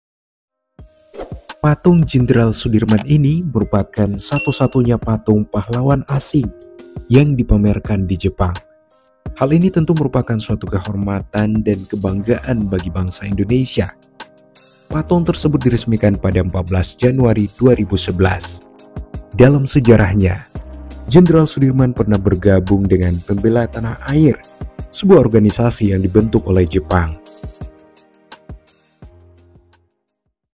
ada patung jenderal soedirman di jepang
Patung Jenderal Sudirman ini merupakan satu-satunya patung pahlawan asing yang dipamerkan di Jepang. Hal ini tentu merupakan suatu kehormatan dan kebanggaan  bagi bangsa Indonesia.
Patung tersebut diresmikan pada 14 Januari 2011,
Dalam sejarahnya, Jenderal Soedirman pernah bergabung dengan Pembela Tanah Air (PETA), sebuah organisasi yang dibentuk oleh Jepang.